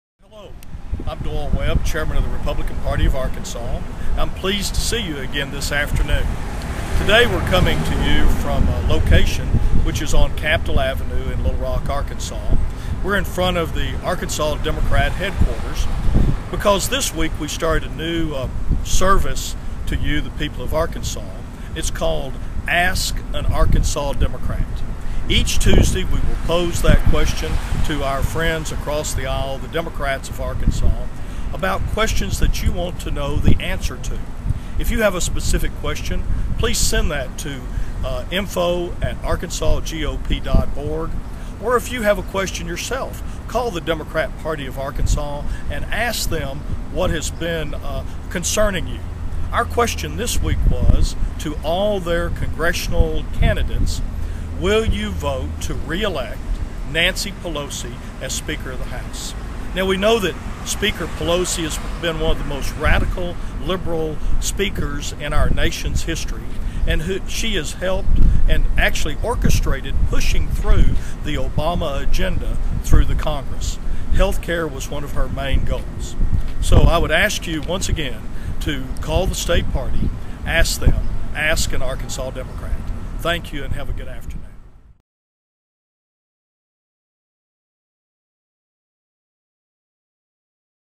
Ask An Arkansas Democrat
This week Chairman Doyle Webb goes on location to introduce the "Ask An Arkansas Democrat" campaign. Each week, the ARGOP will "ask an Arkansas Democrat" a question in order to  demonstrate how Arkansas Democrats are aligned with the liberal agenda of the Washington Democrat Machine.